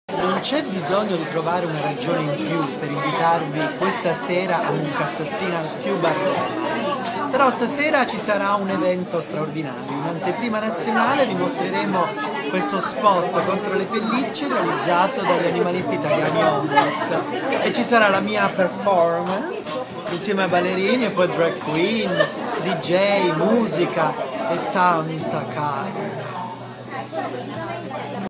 Vladimir Luxuria @ Muccassassina
L'invito di Vladimir Luxuria per Giovedì 5 gennaio. 22esimo compleanno di MUCCASSASSINA